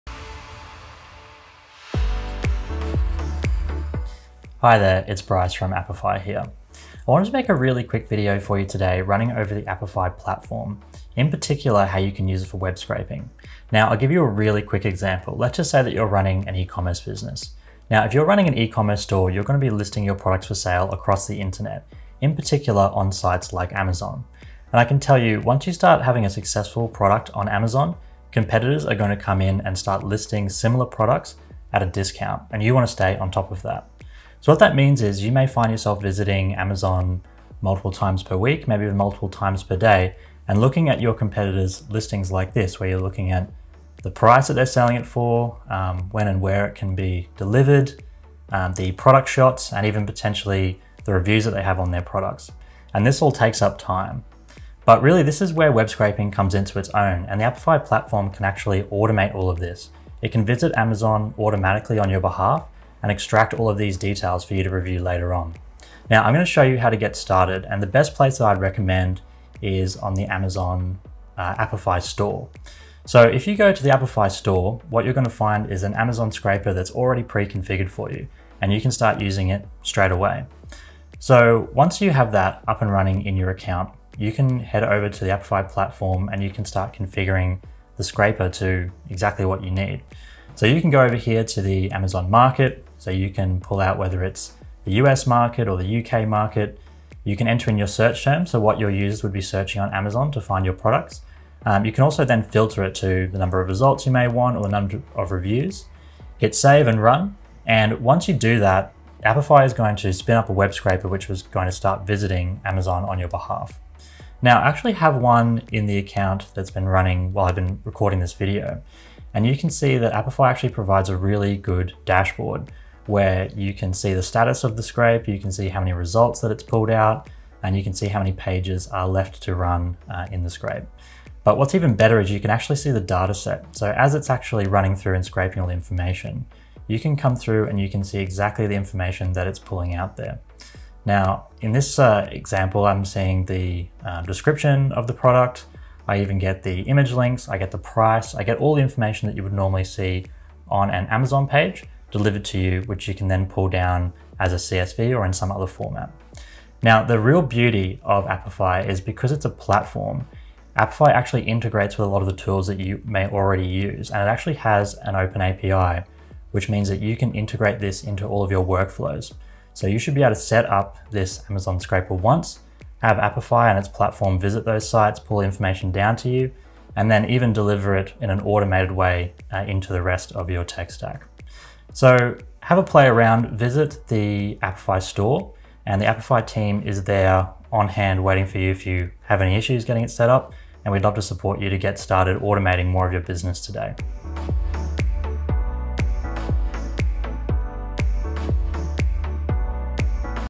How to scrape Amazon to monitor your competitors
Learn how easy it is to use Apify's free web scraping tools to monitor competitor prices on Amazon.
A step-by-step guide to web scraping with Apify. Monitor your competitors on Amazon and automate price matching.

Timestamps:
0:00 - Introduction
1:10 - Apify Store
1:24 - Apify Platform
2:10 - Dataset
2:37 - Integrations
2:30 - Conclusion